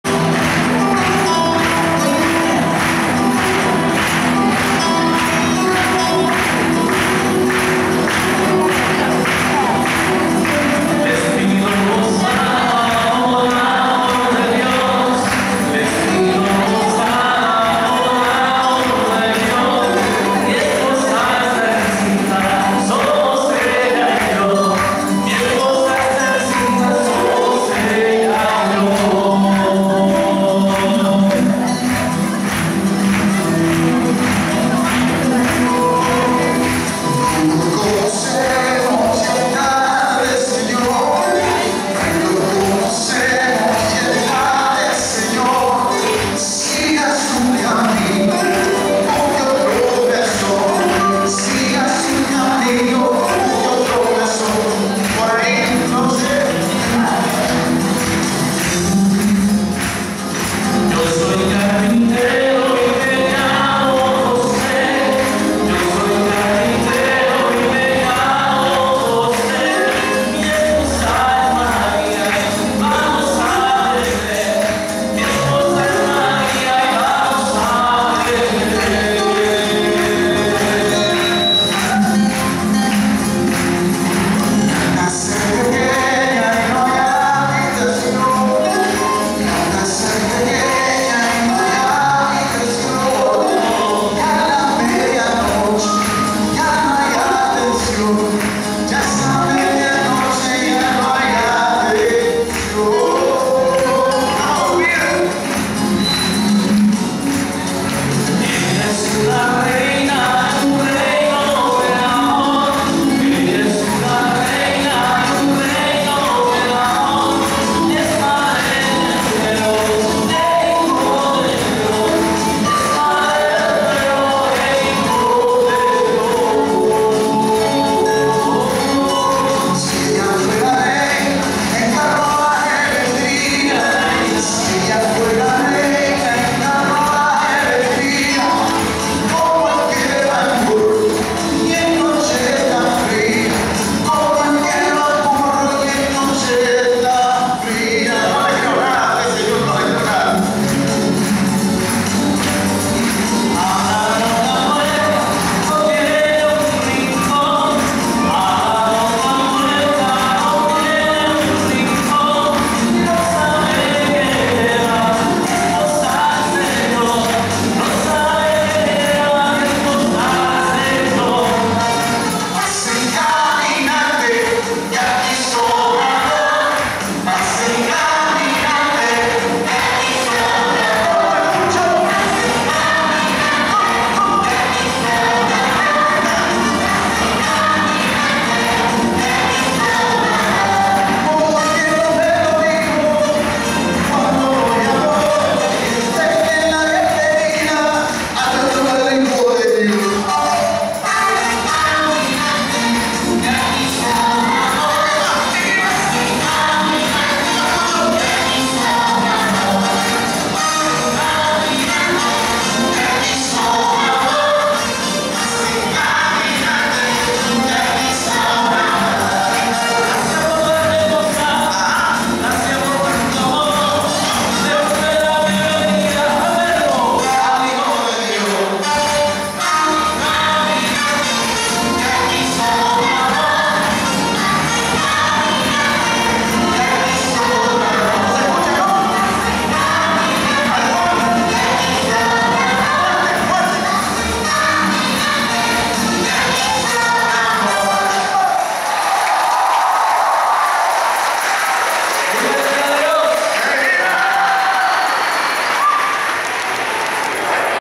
Novena Navidena en CZESTOCHOWA con la participacion de invitados especiales
Septimo dia de la novena en la parroquia con Daniel Betancourt, el grupo coral infantil Borkis y los Padrecitos Jose Manuel y Jorge